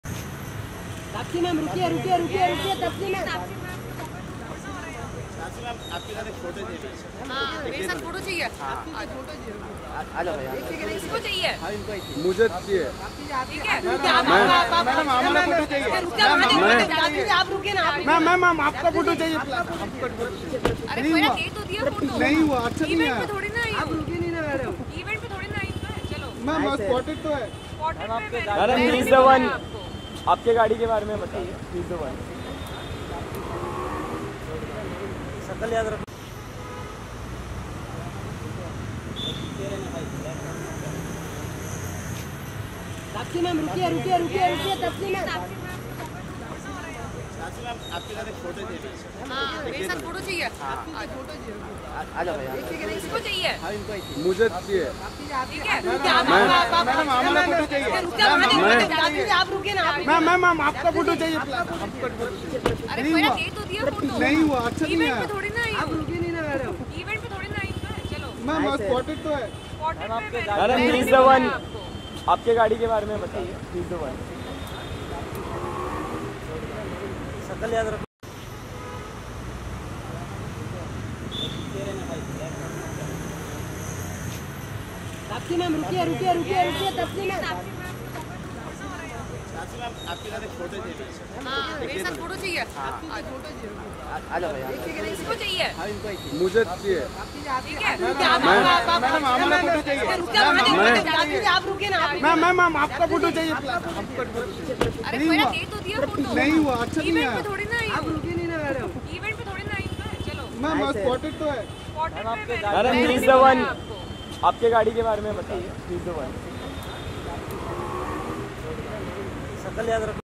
Taapsee Pannu Gets ANGRY With Media Says "I Didn't Call You" | Bollywood 2018
Taapsee Pannu Gets ANGRY With Media Says I Didn't Call You Bollywood Latest Today News Movies Songs Updates 2018